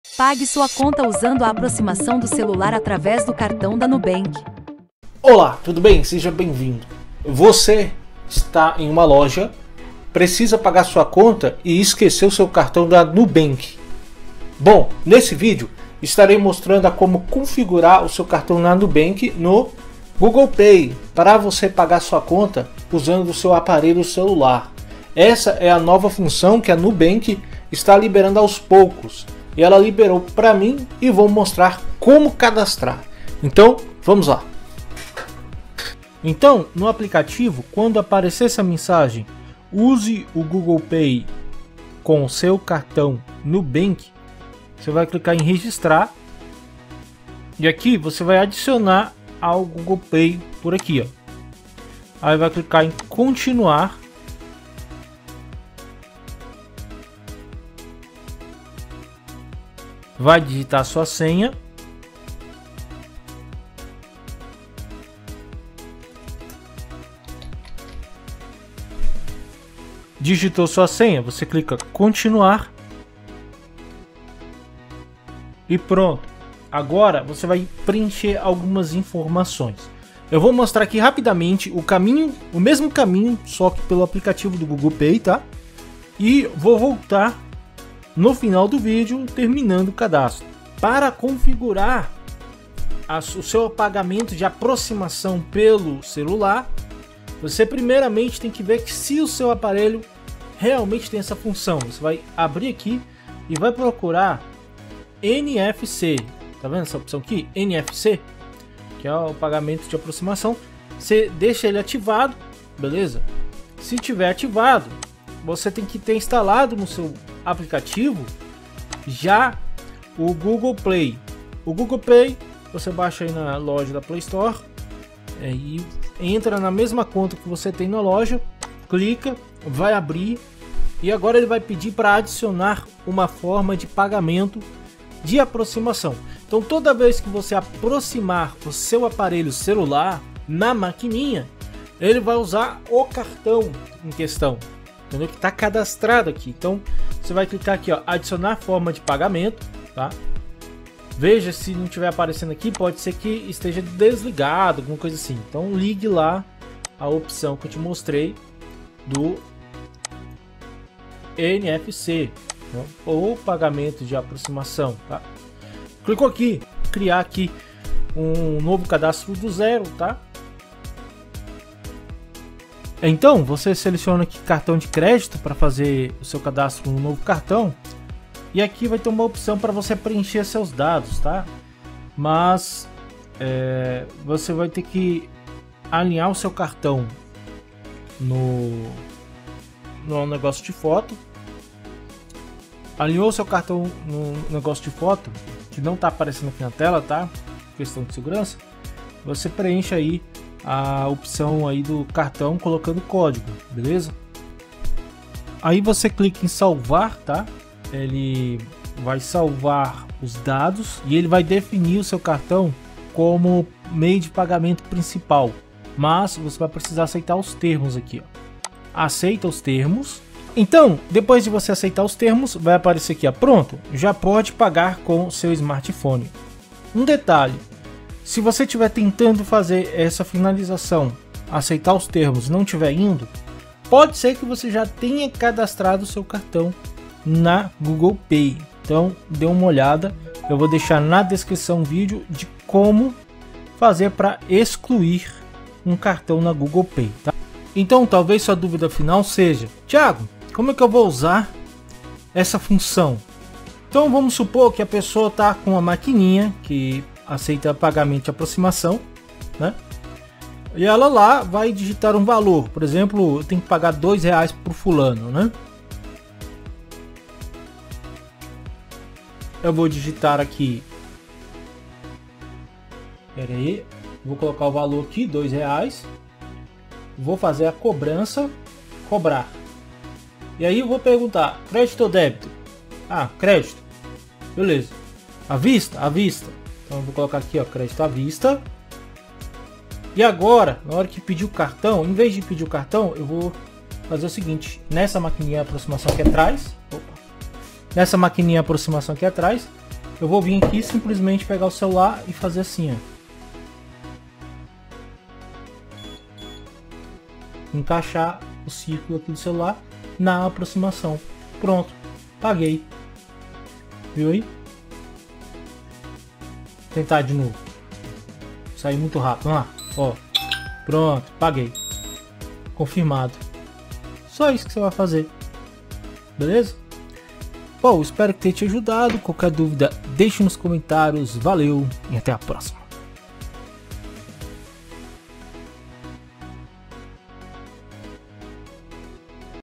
NUBANK NO GOOGLE PAY - chegou! Nubank libera pagamentos com Google Pay/Como cadastrar
Já pensou em pagar sua conta na loja usando o seu celular?
A Nubank agora  está permitindo o cadastro de seu cartão no aplicativo do Google Pay para realizar pagamento com aproximação, veja mais sobre isso no vídeo e veja também como cadastrar o cartão nubank tanto pelo aplicativo da nubank como também pelo Google PAY.
Você também verá sobre
► como adicionar cartão nubank no google pay
► nubank libera pagamentos com google pay
►nubank e google pay
 -Instagram Official: thiagodepolo

►Ajude o canal com 1 real.